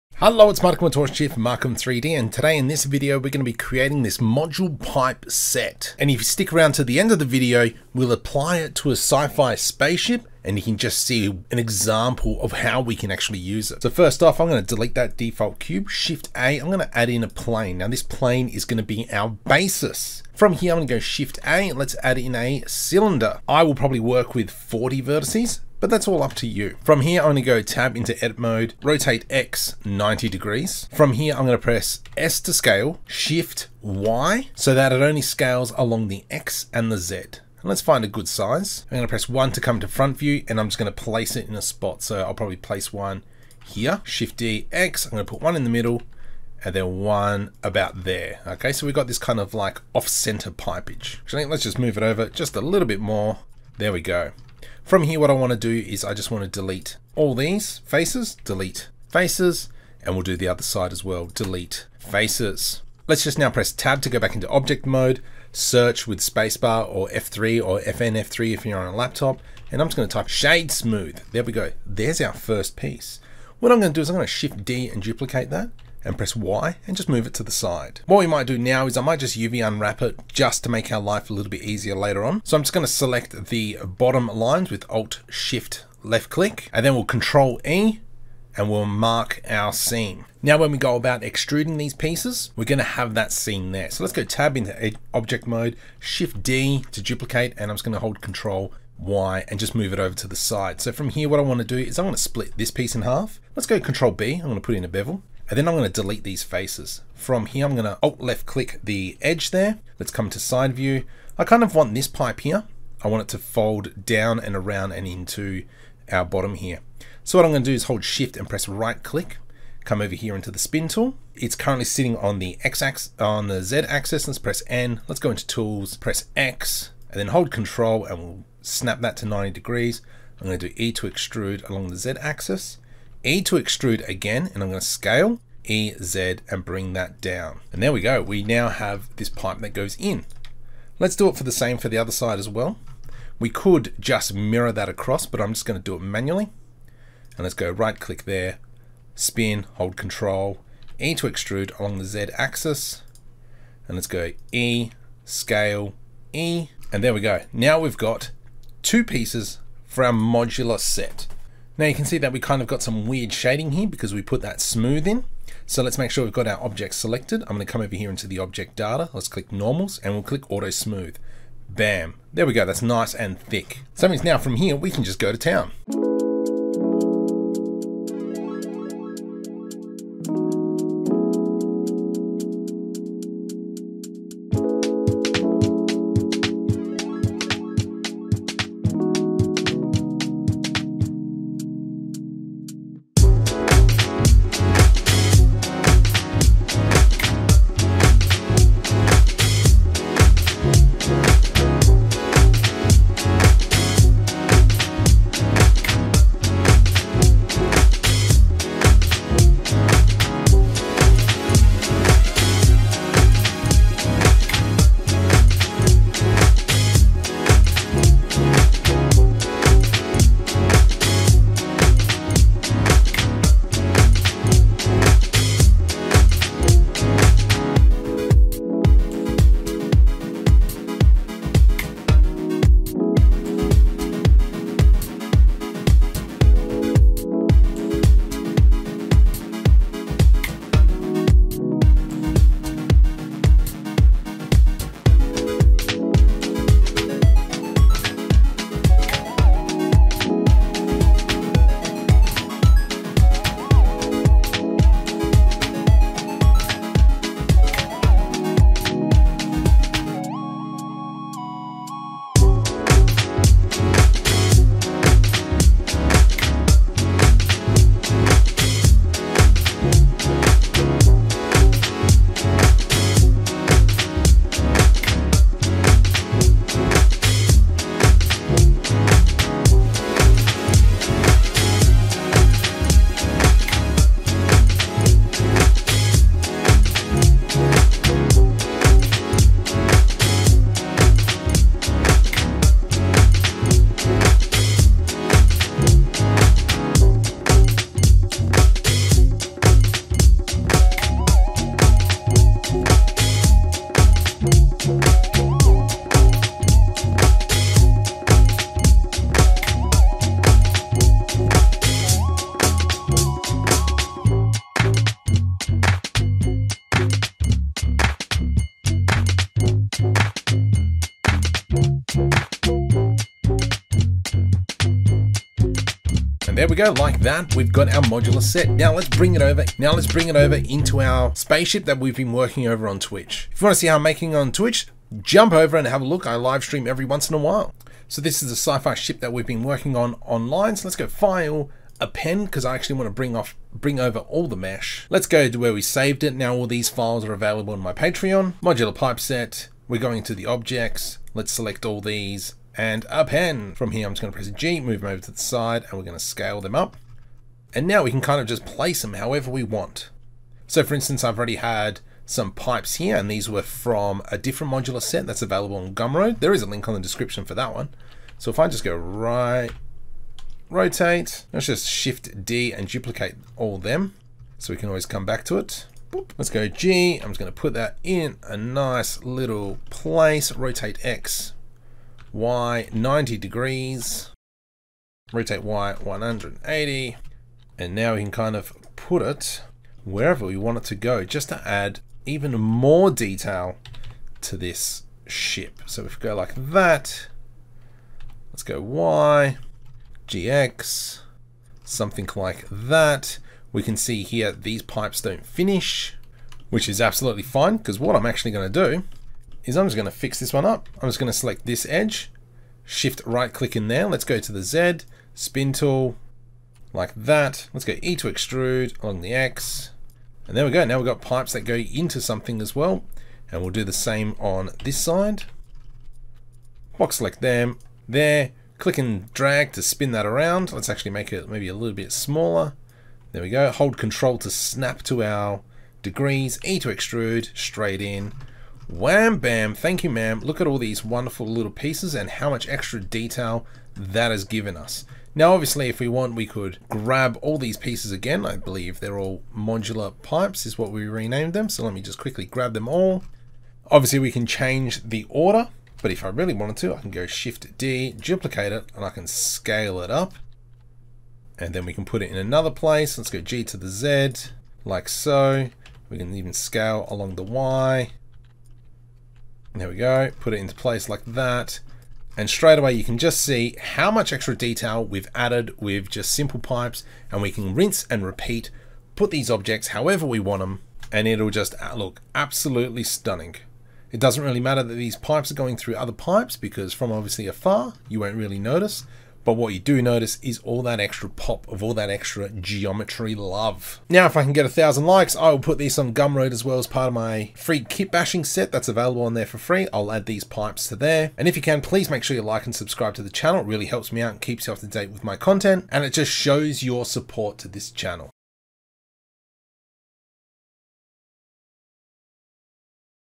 Modular Pipes Asset Creation Blender Tutorial
I create a set of Modular pipes to add to anything for that extra detail. I show you the process that I use to have a set of contentiously flowing pipes. I will also show you how I use them in adding them to a scifi ship

The lighting that I used for the initial scene

The material that I added to the pipes
Extreme PBR Library

The HDRI that I used for the scifi ship render

Once I get 1000 likes, I will add these models to the kitbash link above